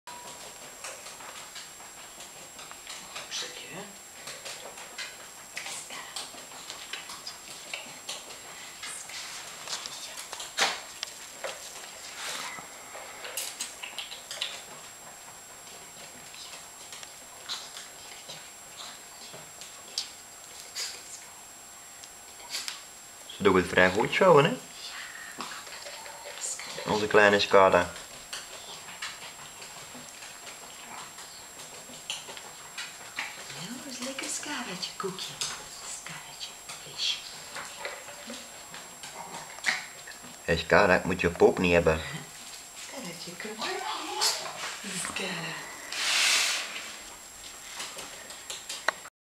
aandacht training voor show honden. Escada, Dina & boy trainen samen met lissa